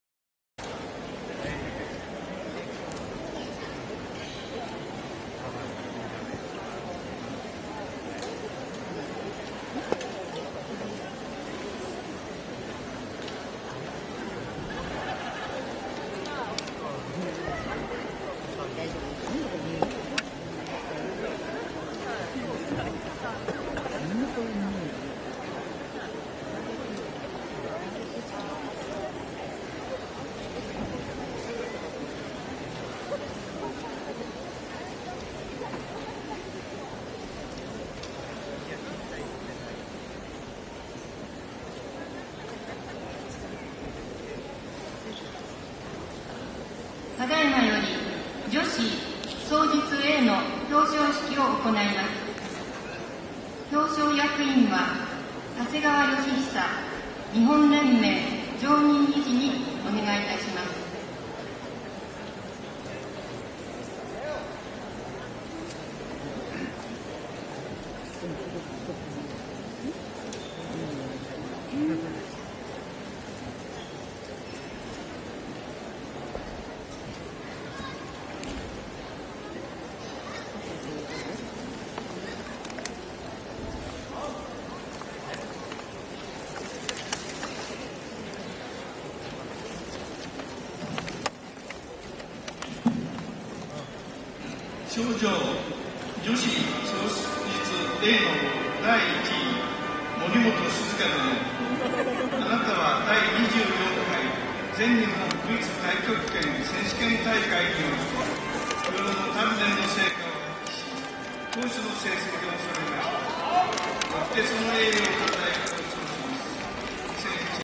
【武術】2007 形意拳 (19/24) / 【Wushu】2007 Xingyiquan (19/24)
競技種目：男子伝統拳術A
Competing event: Men's Traditional Quanshu category A

表演種目：山西形意拳
Performing event: Shanxi Xingyiquan

選手：楊   崇
Competitor: Takashi Yo

男子伝統拳術A第６位
Men's Traditional Quanshu category A 6th place

大会名：第24回全日本武術太極拳選手権大会 (2007年)
Competition name: 24th All Japan Wushu Taijiquan Championship (2007)

第24回全日本武術太極拳選手権大会 (2007年)
24th All Japan Wushu Taijiquan Championship (2007)

第25回全日本武術太極拳選手権大会 (2008年)
男子伝統拳術A第４位
男子伝統器械第５位
25th All Japan Wushu Taijiquan Championship (2008)
Men's Traditional Quanshu category A 4th place
Men's Traditional Weapon 5th place